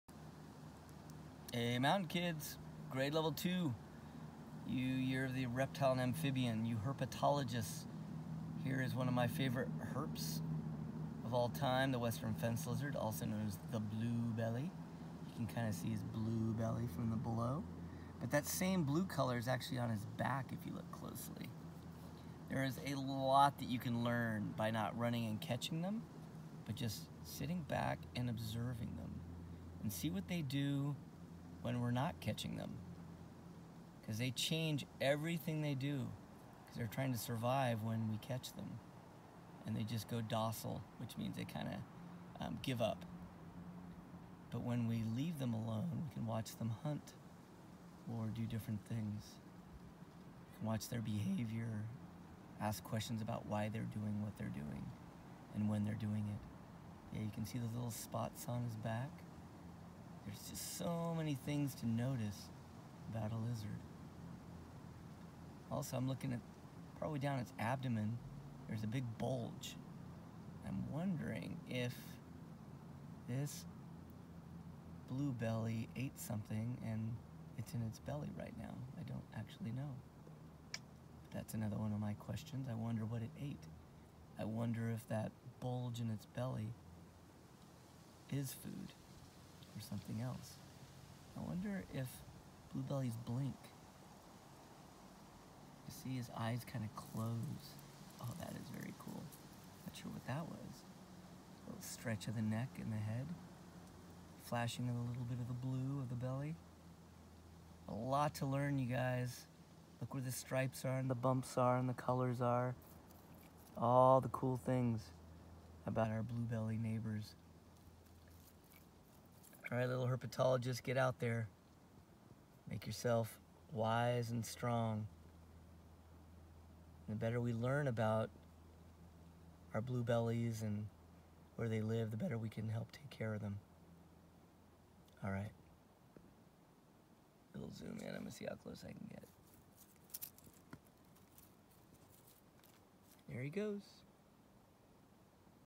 Week 4 - 2nd Grade - Adult Blue Belly Continued and Up Close
Northwestern Fence Lizard is the more accurate name for this reptile living on the south side of my house. Got very close. Look closely. See if you can see enough of its throat to tell if it is a male or female - males have a bright blue throat, females as pale blue throat...